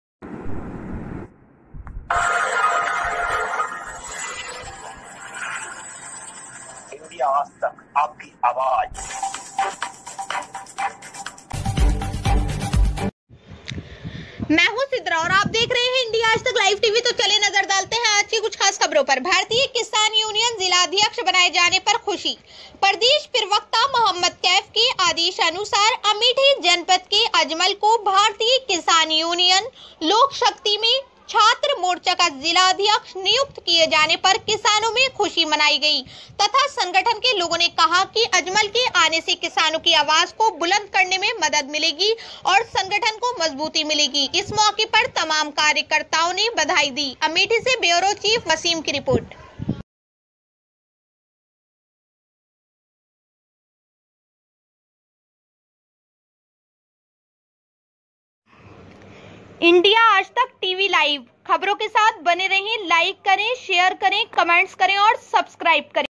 Live भारतीय किसान यूनियन जिलाध्यक्ष बनाये जाने पर खुशी
भारतीय किसान यूनियन जिलाध्यक्ष बनाये जाने पर खुशी

प्रदेश प्रवक्ता मोहम्मद कैफ के आदेशानुसार  अमेठी जनपद के अजमल को भारतीय किसान यूनियन( लोकशक्ति) में छात्र मोर्चा का जिलाध्यक्ष नियुक्त किए जाने पर किसानों में खुशी मनाई गई । तथा संगठन के लोगों ने कहा कि अजमल के आने से किसानों की आवाज को बुलंद करने में मदद मिलेगी और संगठन को मजबूती मिलेगी इस मौके पर तमाम कार्यकर्ताओं ने बधाई दी।

अमेठी से ब्यूरो चीफ वसीम की रिपोर्ट